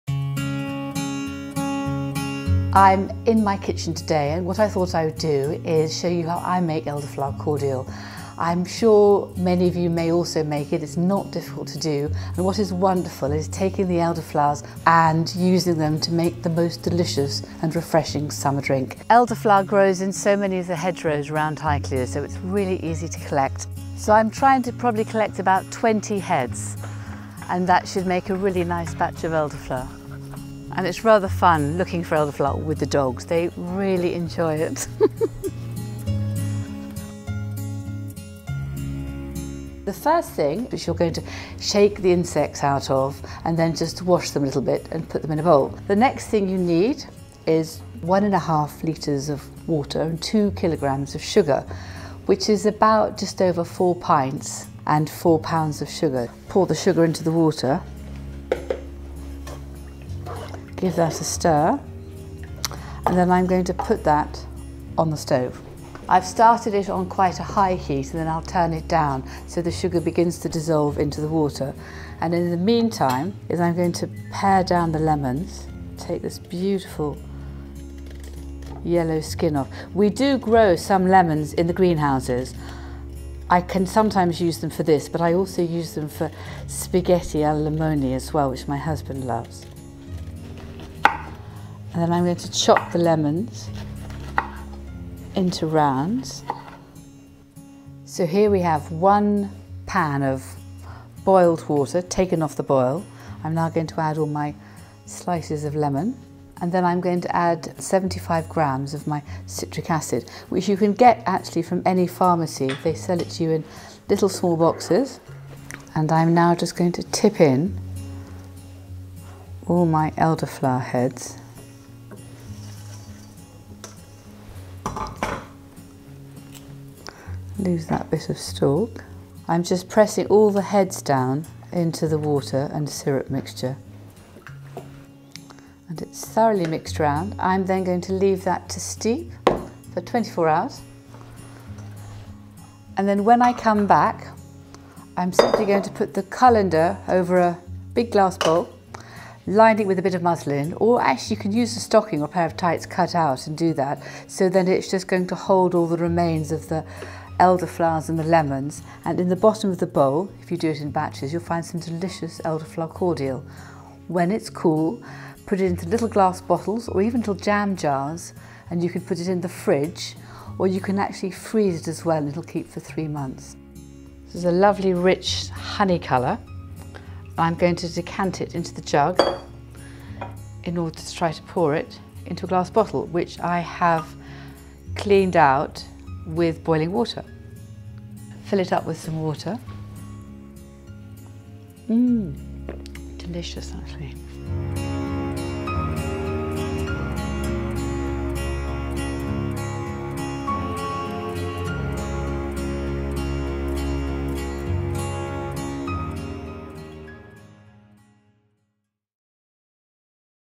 Cooking with Lady Carnarvon - Homemade Elderflower Cordial
The summer weather is here and it's a perfect opportunity to sit back and relax with a nice refreshing glass of homemade elderflower cordial.